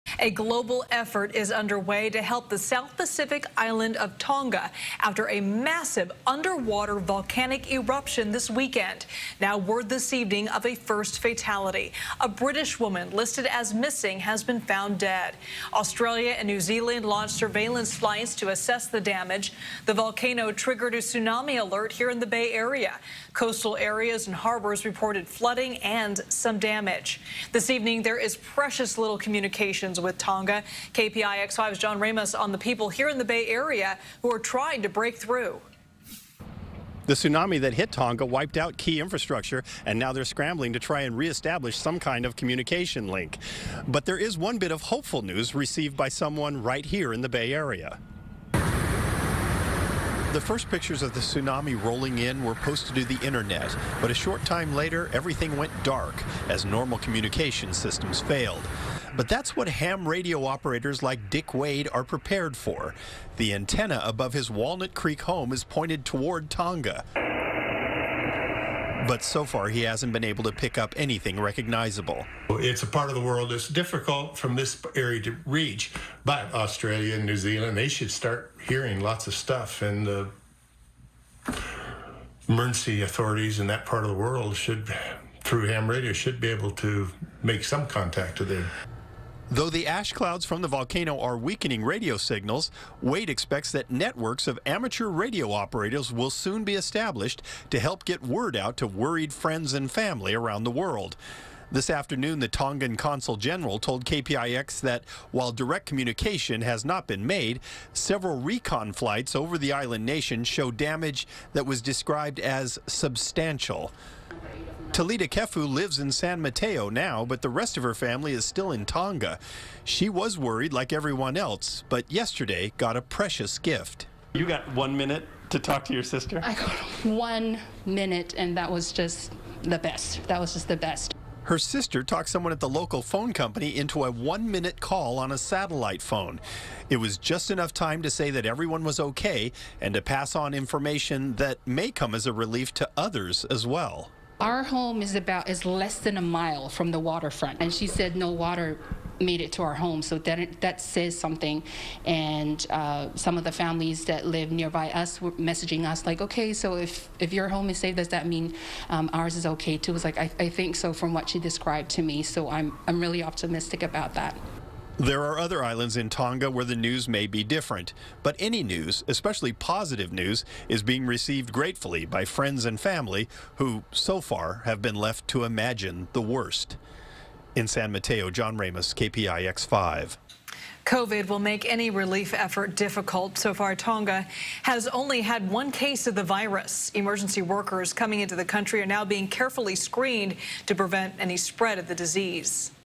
Bay Area Relatives Get Message Of Hope From Tonga
A family got a message of hope from Tonga after massive underground volcano triggered a tsunami and wiped out the island nation's communications infrastructure. John Ramos talked to Bay Area HAM radio operator trying to help.